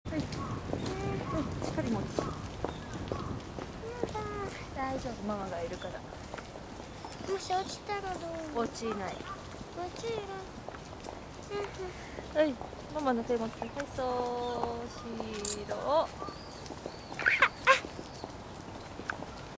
Captured by the camera sitting on a railing
Here is Chidorigafuchi where cherry trees is full bloom. My kids is stting on a railing and captured by the camera. He says "It's scary!"